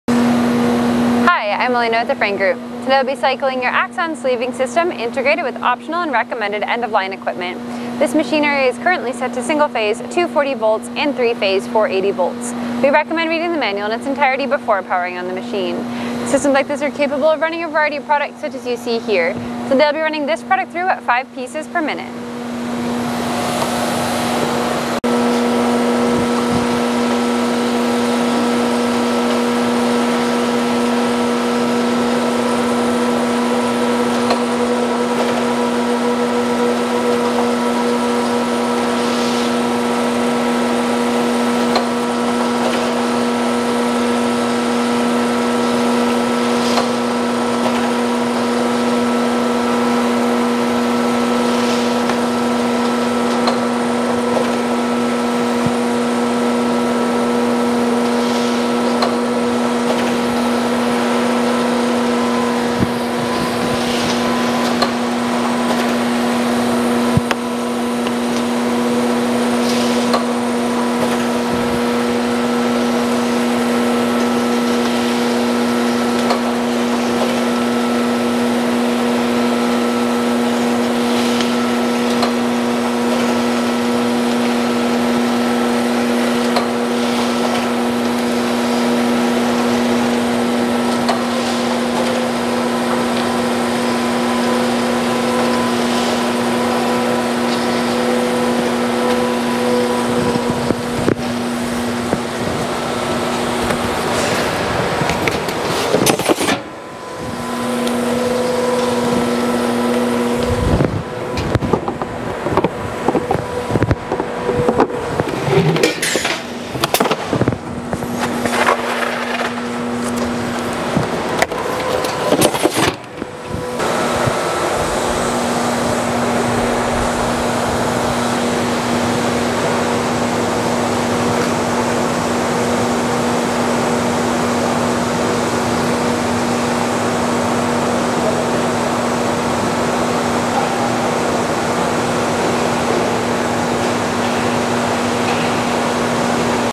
Axon EZ130 Sleever Demonstration
Here we're running one of our Axon EZ130 Sleever Demonstrations.

Axon, Model EZ-130, automatic, tamper evident shrink neck bander or sleeve label applicator with Model EZ-48 shrink tunnel. Neck bander equipped with photo eyes for machine activation, film registration, stepper drive film feed system, tamp band pusher, guillotine style cutter with durable and dependable (2) blade cutting system. Layflat: 18mm to 130mm. Container diameter: 10mm to 79mm. Film thickness 40 to 70 microns. Film specs: up to 15" OD roll on 4-7/8" OD core Control panel with touch screen HMI controller with safety door guarding and e-stop. Mounted on heavy duty frame.

Shrink tunnel has 12" high x 7½" wide aperture x 48" long chamber, digital heat controls and pneumatic tunnel lift. System mounted above 6" wide x 176" long stainless steel table top belt conveyor with spacing wheel and variable speed control. Mounted on (4) leg frame.

Speed dependent on materials, application and machine configuration.